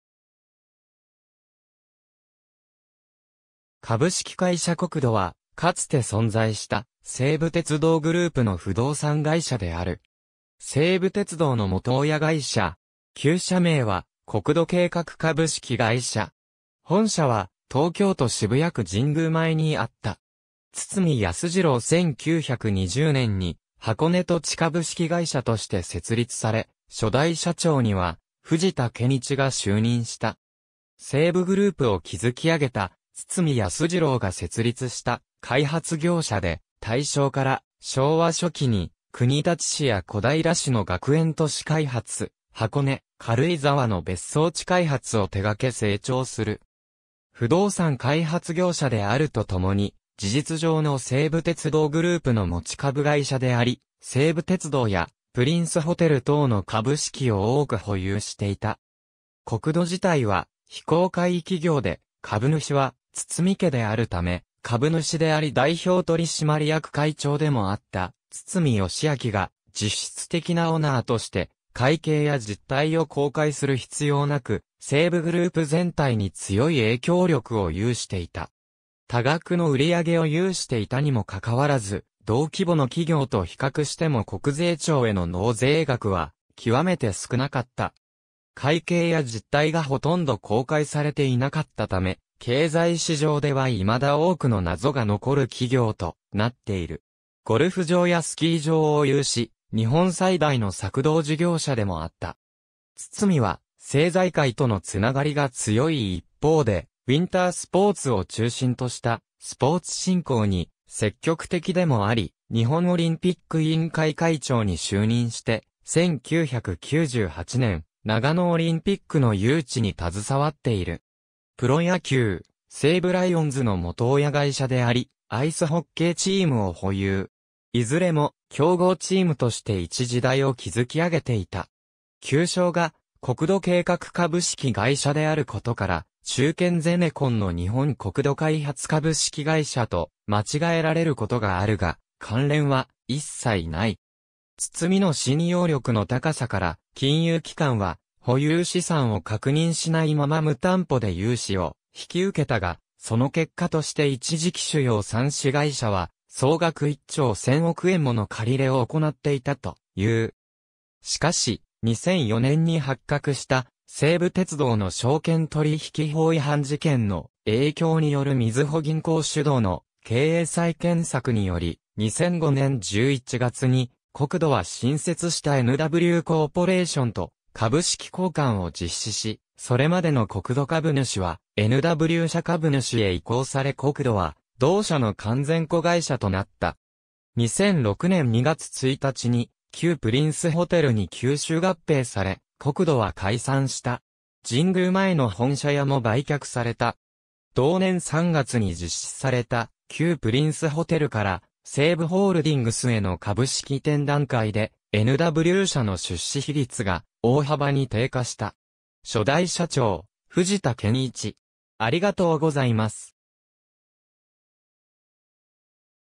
コクド
神宮前_(渋谷区)
株式会社コクドは、かつて存在した西武鉄道グループの不動産会社である。
西武鉄道の元親会社。
旧社名は国土計画株式会社。
本社は東京都渋谷区神宮前（原宿駅前）にあった。
堤康次郎 1920年に箱根土地株式会社として設立され、初代社長には藤田謙一が就任した。
西武グループを築きあげた堤康次郎が設立した開発業者で、大正から昭和初期に国立市（国立駅）や小平市（一橋学園）の学園都市開発、箱根（仙石原など）・軽井沢（千ヶ滝・南軽井沢・レイクニュータウンなど）の別荘地開発を手掛け成長する。
不動産開発業者であるとともに、事実上の西武鉄道グループの持株会社であり、西武鉄道やプリンスホテル等の株式を多く保有していた。
コクド自体は、非公開企業で株主は堤家であるため、株主であり代表取締役会長でもあった堤義明が実質的なオーナーとして、会計や実態を公開する必要なく、西武グループ全体に強い影響力を有していた。
多額の売り上げを有していたにも関わらず、同規模の企業と比較しても国税庁への納税額は極めて少なかった。
会計や実態がほとんど公開されていなかったため、経済史上では未だ多くの謎が残る企業となっている。
ゴルフ場やスキー場を有し、日本最大の索道（リフト・ゴンドラ・ロープウェイ）事業者でもあった。
堤は政財界との繋がりが強い一方で、ウィンタースポーツを中心としたスポーツ振興に積極的でもあり、日本オリンピック委員会会長に就任して、1998年長野オリンピックの誘致に携わっている。
プロ野球・西武ライオンズの元親会社（吸収によって2008年までプリンスホテルが親会社。後述の通り2009年以降西武ホールディングスへ経営権が移動）であり、アイスホッケーチームを保有。
いずれも強豪チームとして一時代を築き上げていた。
旧称が国土計画株式会社であることから、中堅ゼネコンの日本国土開発株式会社と間違えられることがあるが、関連は一切ない（関連会社としては西武建設がある）。
堤の信用力の高さから、金融機関は保有資産を確認しないまま無担保で融資を引き受けたが、その結果として一時期主要3子会社は総額1兆1000億円もの借り入れを行っていたという。
しかし、2004年に発覚した西武鉄道の証券取引法違反事件の影響によるみずほ銀行主導の経営再建策により、2005年11月にコクドは新設したNWコーポレーションと株式交換を実施し、それまでのコクド株主（堤家）はNW社株主へ移行されコクドは同社の完全子会社となった。
2006年2月1日に旧プリンスホテル（西武鉄道子会社）に吸収合併され、コクドは解散した。
神宮前の本社屋も売却された。
同年3月に実施された旧プリンスホテルから（機関投資家を相手とする第三者割当増資を実施した）西武ホールディングスへの株式移転段階で、NW社の出資比率が大幅に低下した。
初代社長・藤田謙一